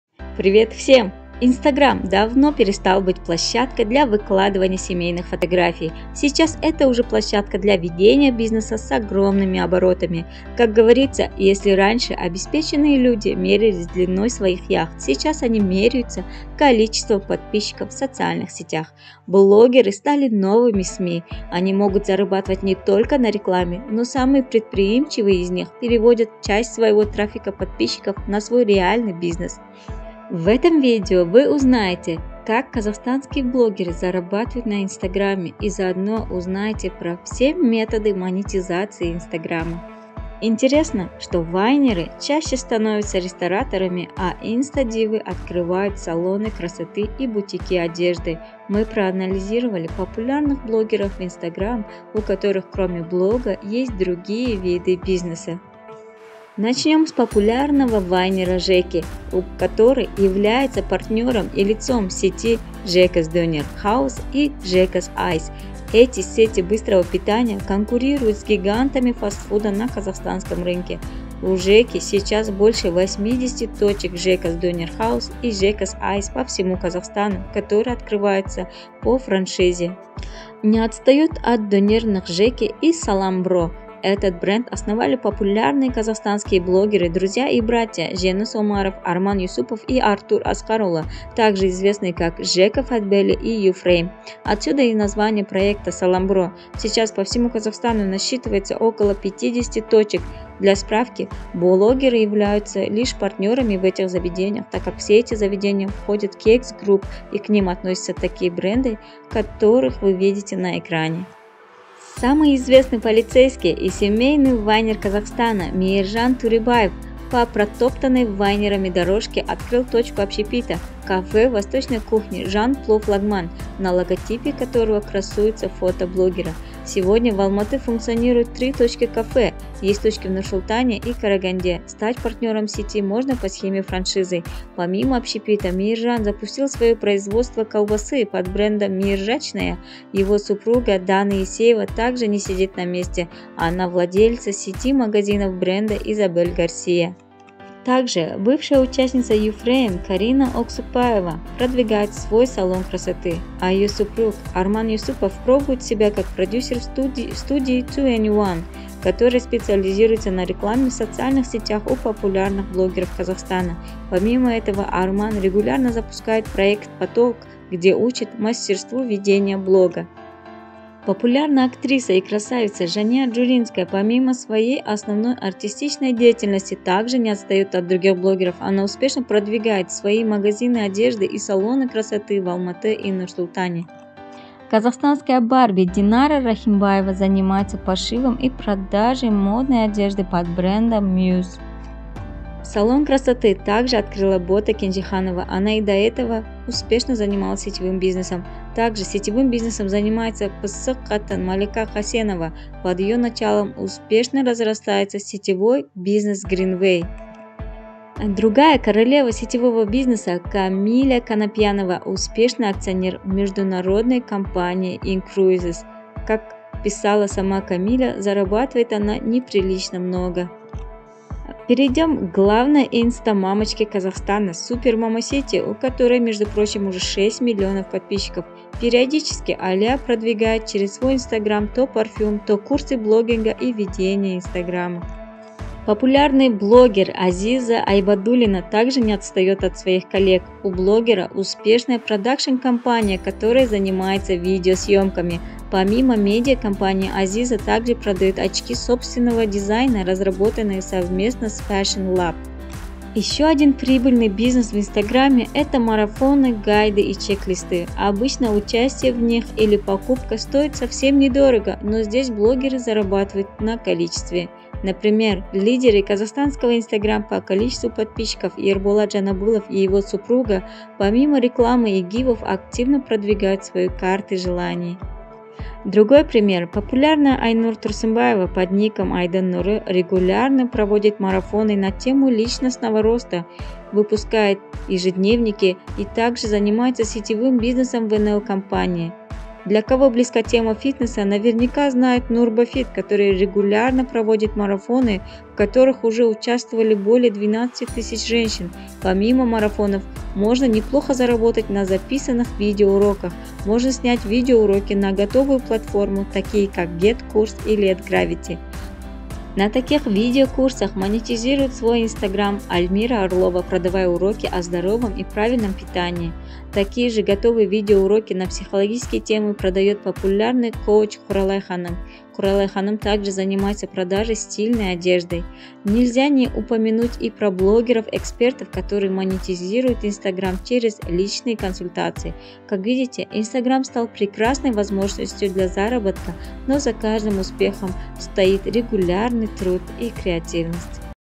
Как зарабатывают на своём Инстаграме ТОПовый БЛОГЕРы КАЗАХСТАНА! Узнай про методы монетизации!
Развлекательное видео про методы монетизации наших казахстанских блогеров Инстаграма!